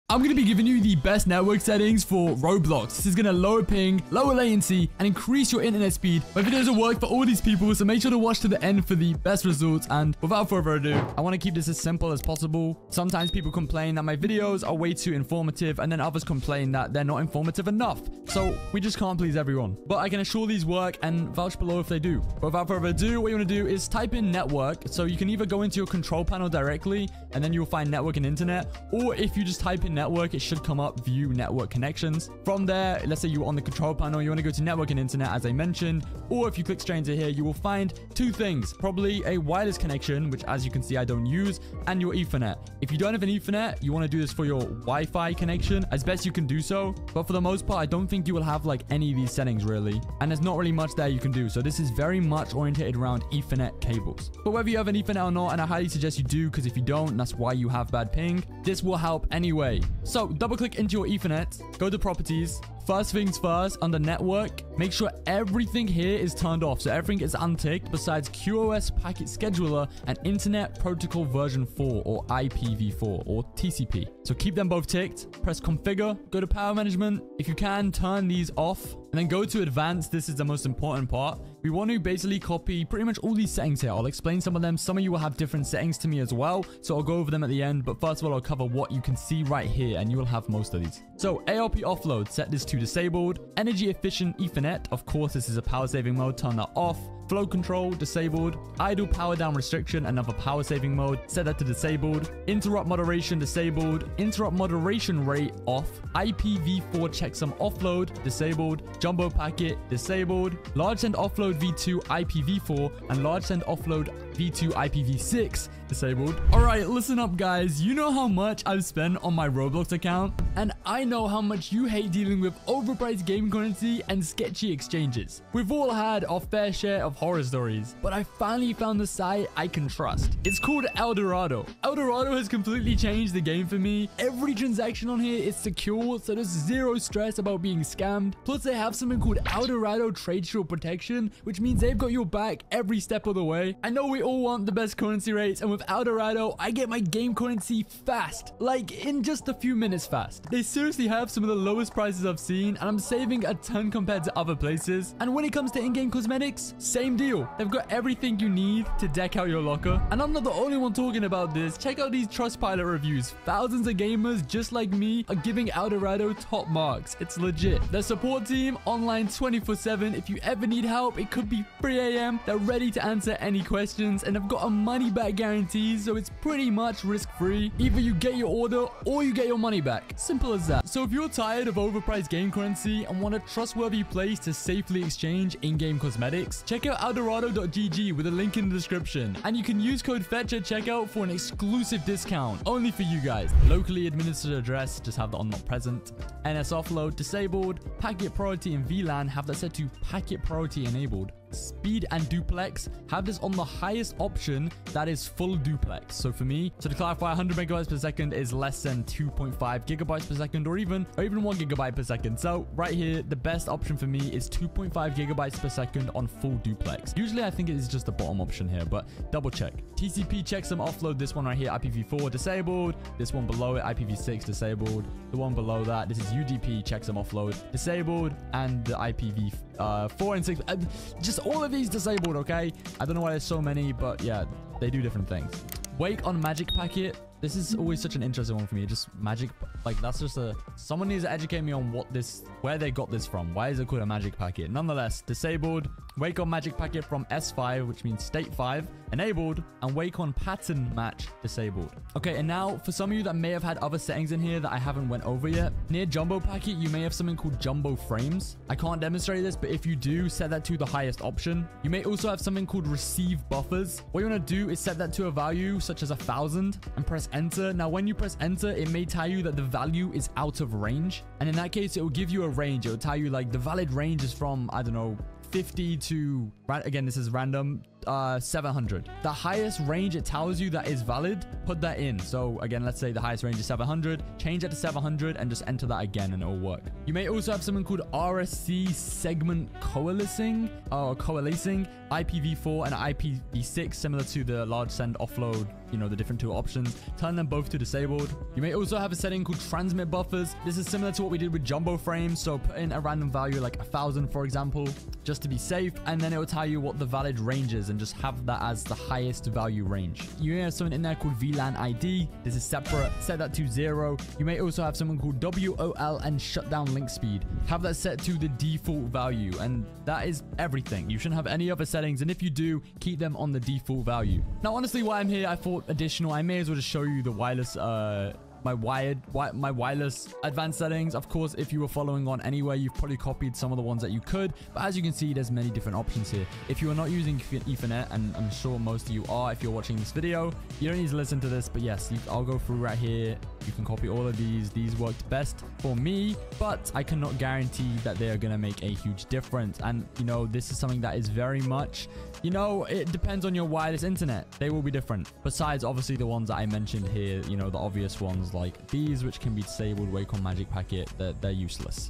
BEST Network Settings for Roblox - LOW PING & FIX LAG 2025
Sick of high ping and lag ruining your Roblox gameplay? Discover the BEST Roblox internet and Ethernet settings for 2025 to get ultra low ping, fix lag & stuttering, and optimize your network adapter for flawless gaming. Whether you're using Windows 10 or 11, this guide shows how to tweak your Ethernet settings, pick the top gaming cables, and apply pro-level network optimizations that work for Roblox, Fortnite, and any PC or mobile device. Say goodbye to lag spikes, input delay, and jitter with fast, stable connections using the latest 2025 Ethernet drivers, DNS tweaks, and powerful network adapter tweaks. Perfect for low-end PCs and mobile, plus bonus tips on choosing the fastest Ethernet cables for gaming and reducing ping on Roblox servers instantly!

Comment "1 MILLION ROBUX PLS" if you're still reading this so I can notice you 👀